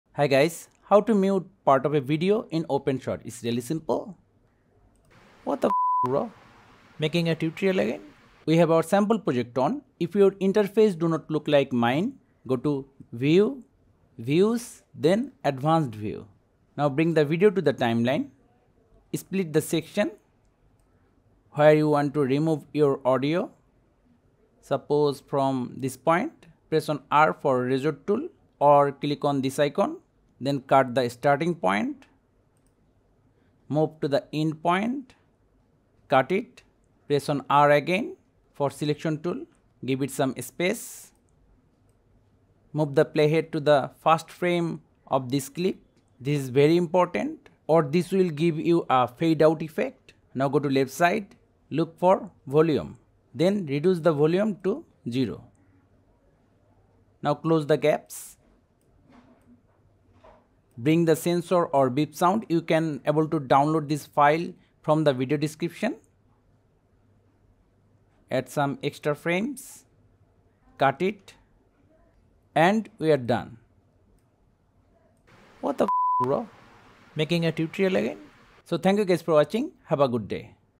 Mute or Censor part of a Video with Beep sound | OpenShot tutorial
In this tutorial, I will show you how to censor words in a video or audio clip by adding a beep sound or muting in OpenShot. This is a beginner-friendly tutorial on OpenShot video editing.